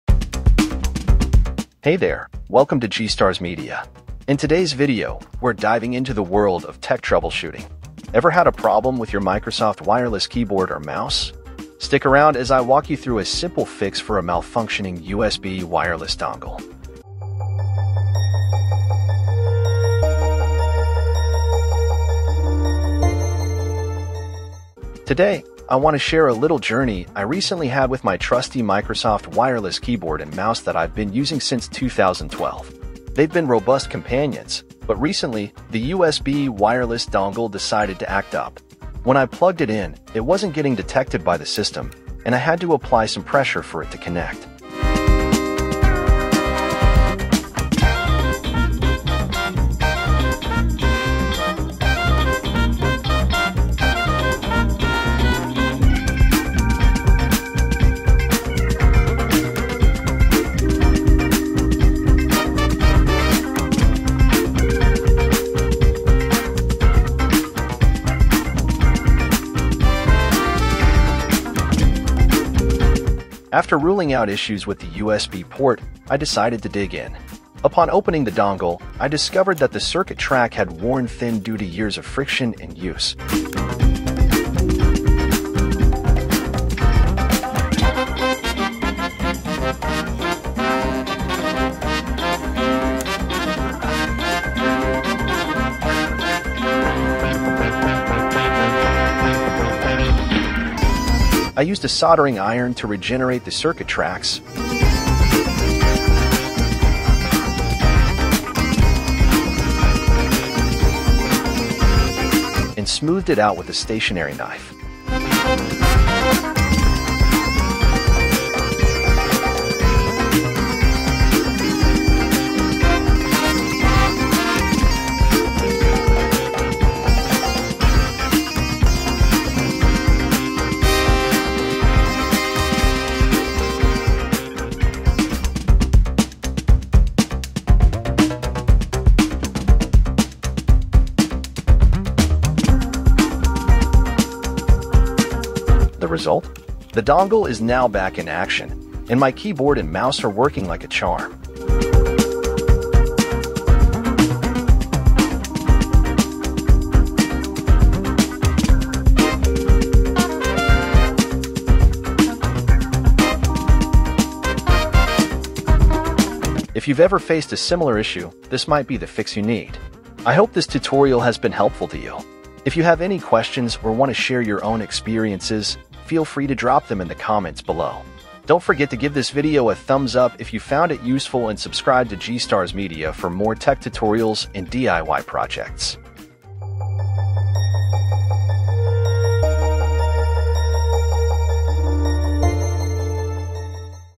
Wireless Keyboard USB Dongle: Fix it in easy steps
Learn how to troubleshoot and repair a malfunctioning USB wireless dongle for your Microsoft wireless keyboard and mouse in this DIY tutorial. Watch as we tackle the issue step-by-step, regenerating the circuit track to restore functionality. Never let a faulty dongle hold you back again!